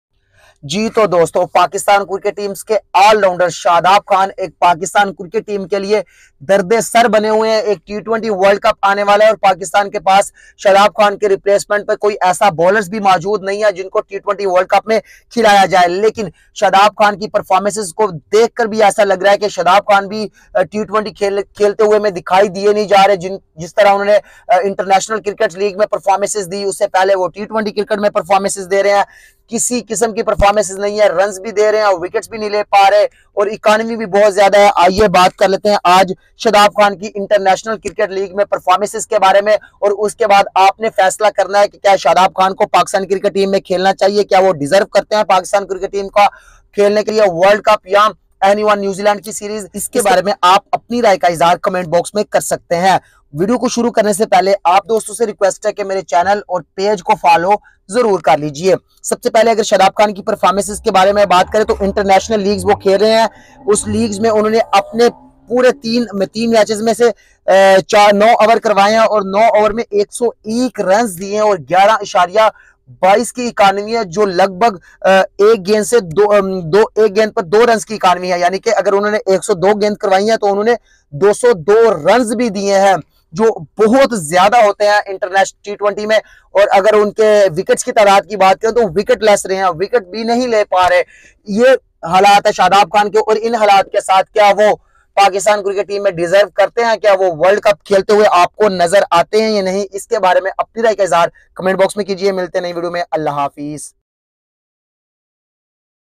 Hamry Shadab Khan Kahan Gum Ho Gya?Pakistan Team Ko Shadab Khan Ki Bhot Zurty hain Shadab Wapis Ajao
Tiwtter